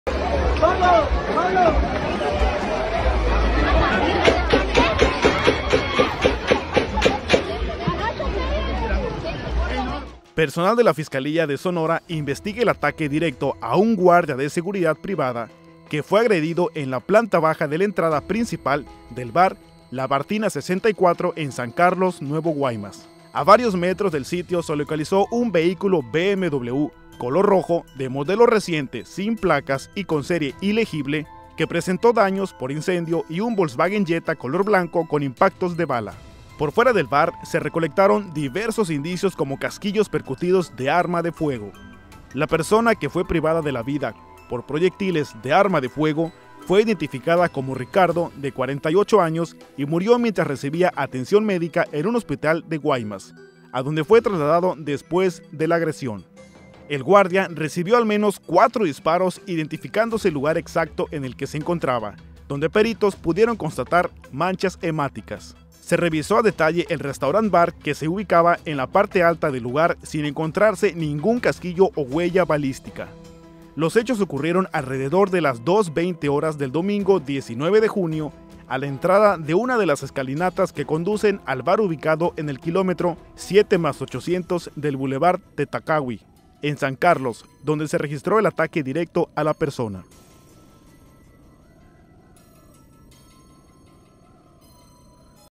Balacera en Guaymas Sonora
Balacera en Guaymas Sonora causa terror durante la noche